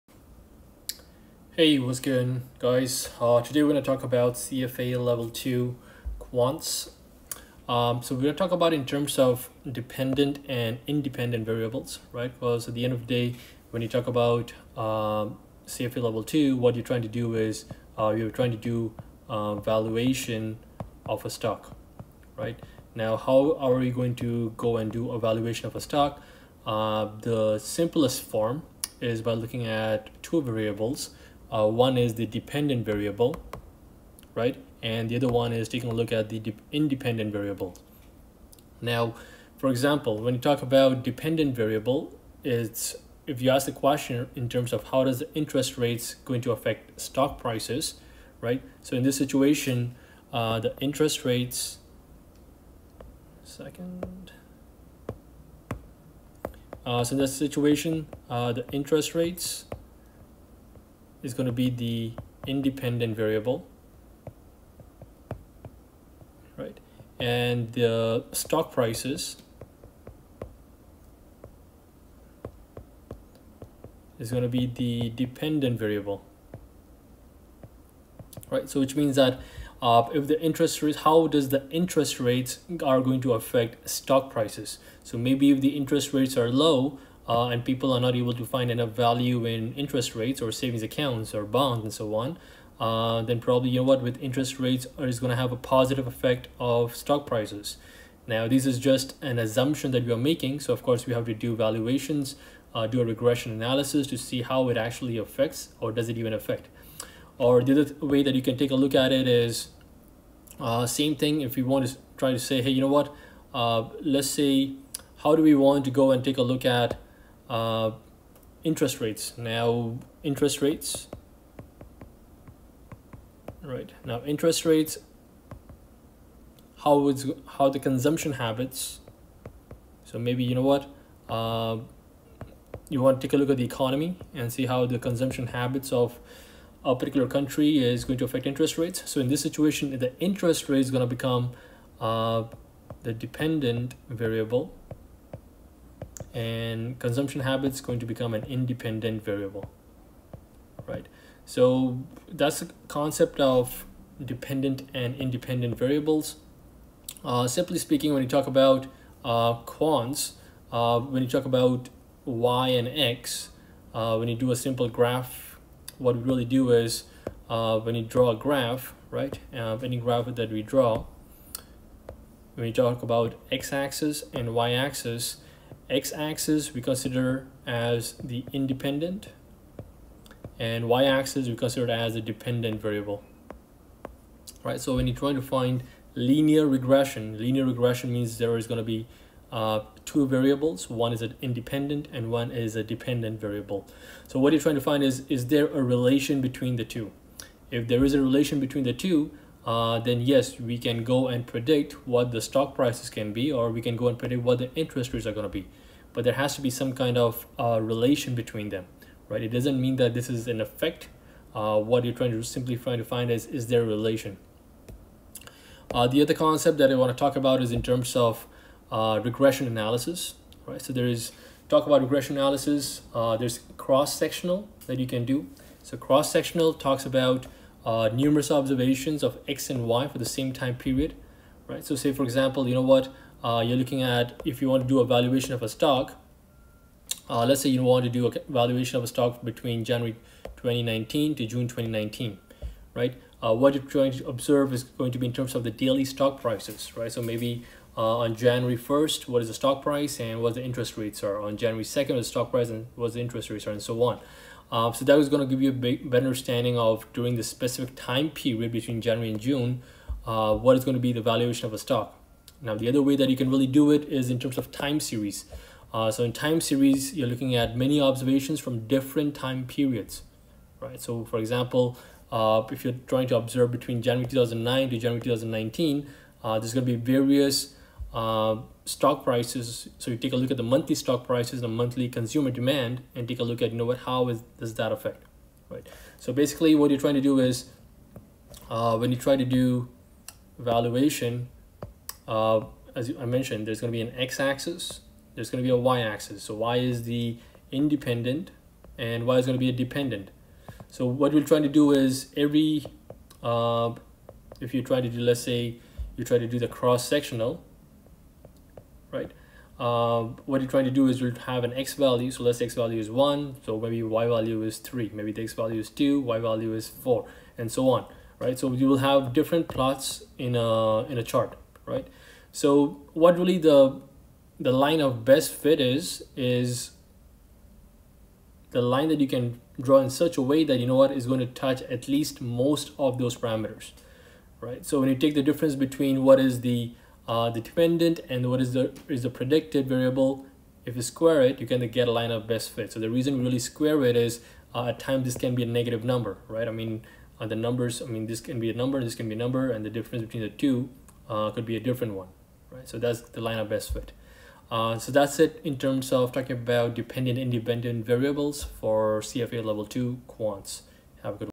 CFA Level 2 2020: Quantitative Methods- Dependent and Independent Variable
This video is targeted towards CFA Level 2 2020 candidates preparing for CFA Level 2 2020:

Quantitative Methods- Dependent and Independent Variable

Linear Regression helps understand the relationship between an independent variable (X) and a dependent variable (Y).
Example 1: How does interest rates effect stock Prices? ( Interest Rates is independent variable and Stock Prices is dependent variable)

Example 2: How does consumption habits effect interest rates? (  Consumption habits is independent variable and interest rates is dependent variable)

Regression Analysis
Cross Sectional: numerous observations of X and Y for same time period
Time Series: Many Observations from different time periods.